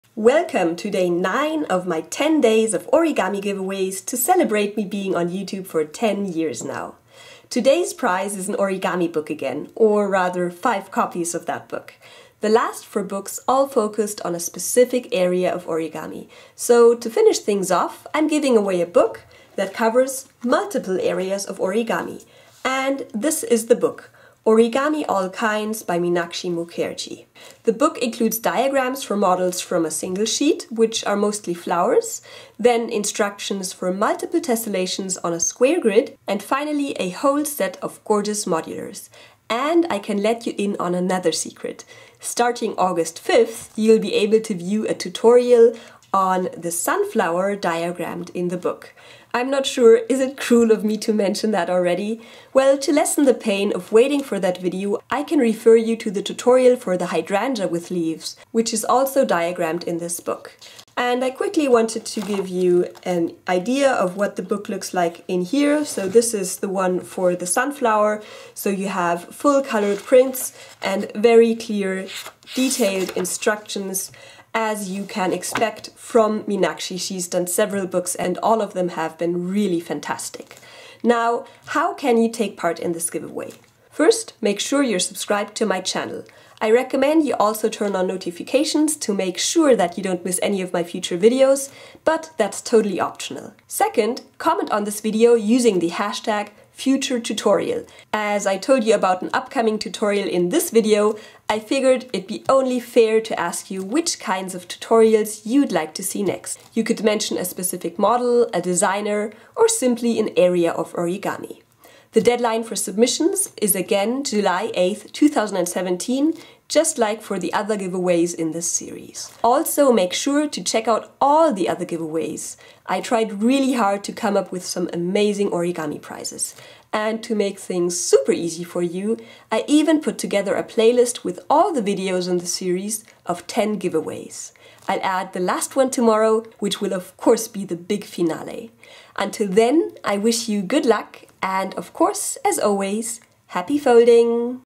[past] Origami Giveaway Day 9 of 10 (Celebrating 10 years on YouTube)
265 of you participated and you had many great suggestions. The top three were of course more general ones: tessellations (72), animals (40), and flowers (28). Thanks for giving me many ideas for future tutorials, as well as letting me know which areas of origami you enjoy most!

To take part:
2. Comment on this video using the hashtag futuretutorial and let me know which kinds of tutorials you want to see from me next. You could mention a specific model, a designer, an area of origami, etc.
Deadline: July 8th 2017

To celebrate my 10-year YouTube anniversary and to thank you for all your support throughout the years, I'm doing 10 days of giveaways and this is the ninth day!

Today's prize: 5 copies of the book "Origami All Kinds" by Meenakshi Mukerji
You can read more about the book (including where to buy it)

Playlist of all videos in my 10 days of giveaways to celebrate my 10-year YouTube anniversary